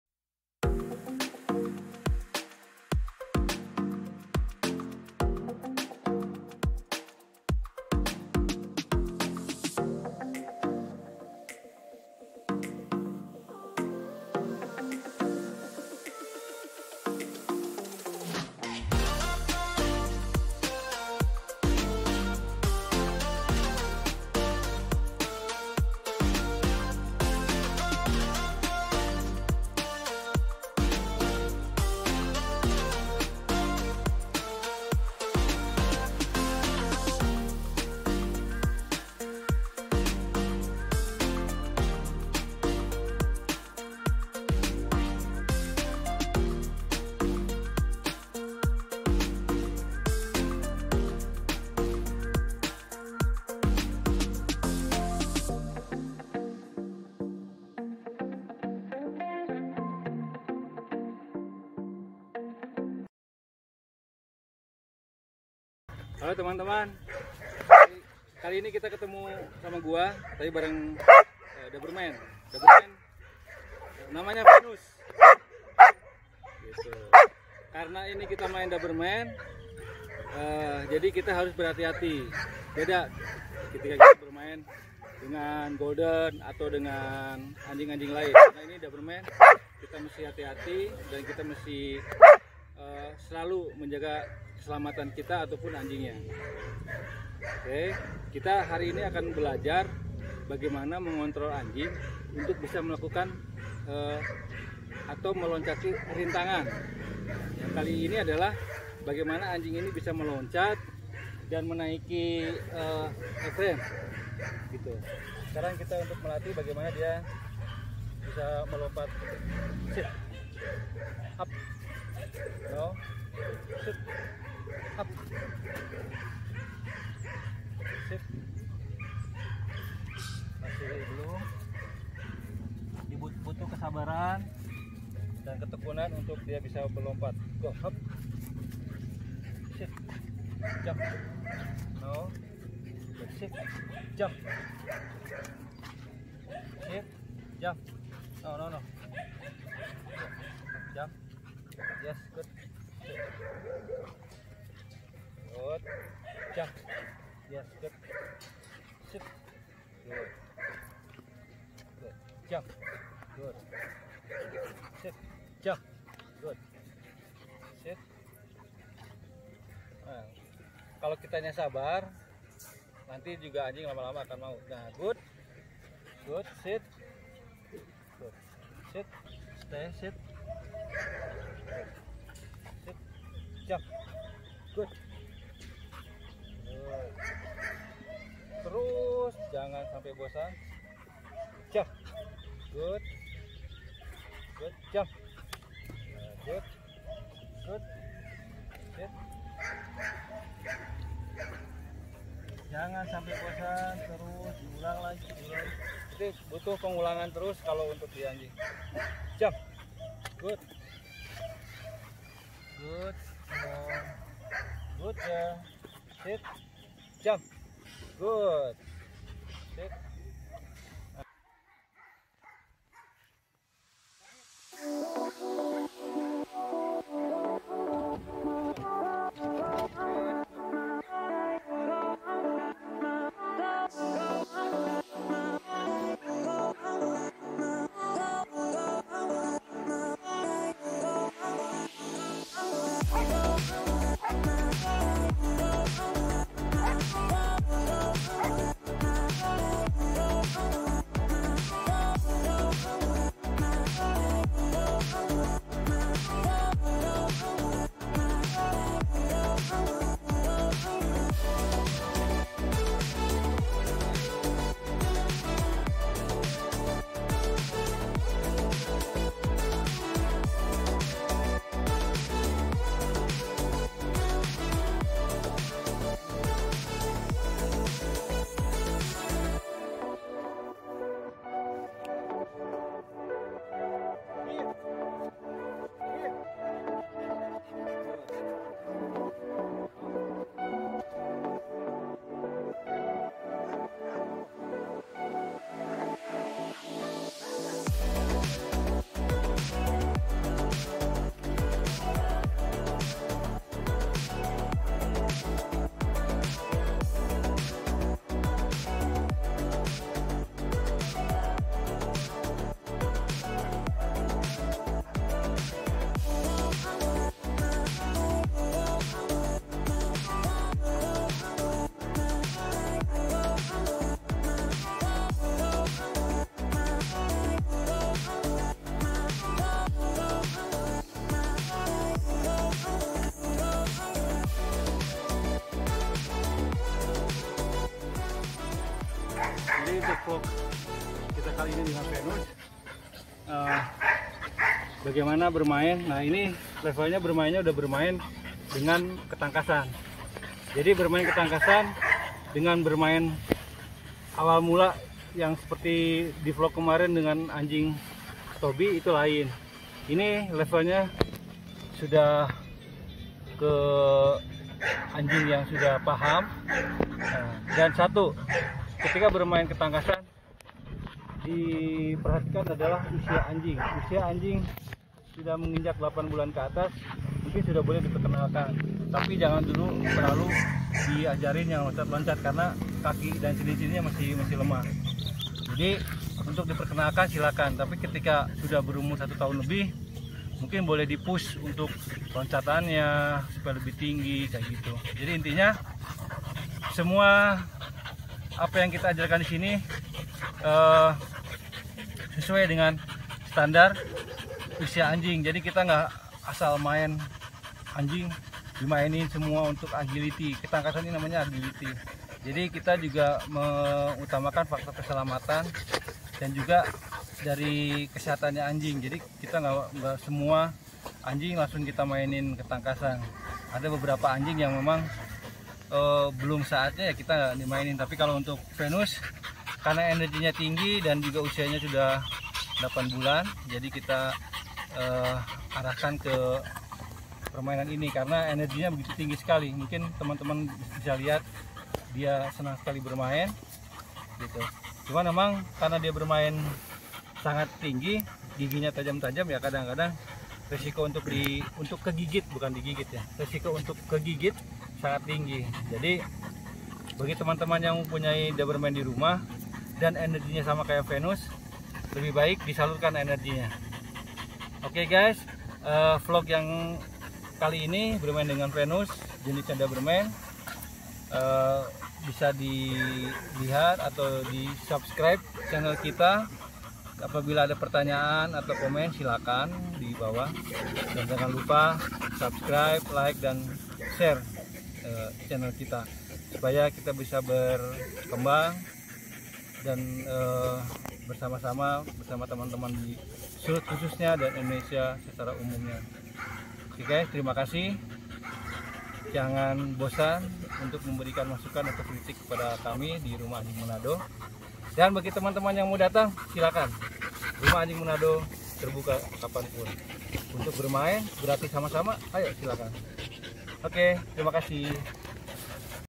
ANJING HARUS JADI SAHABAT, PELINDUNG KITA DAN KELUARGA
Energi yg tinggi dari jenis ini kadang membuat kita harus berhati2 dalam memberikan pelstihan dan treatment dari tangan sering kali tangan kita tergigit karena drive afau pergerakan anjing yg tinggi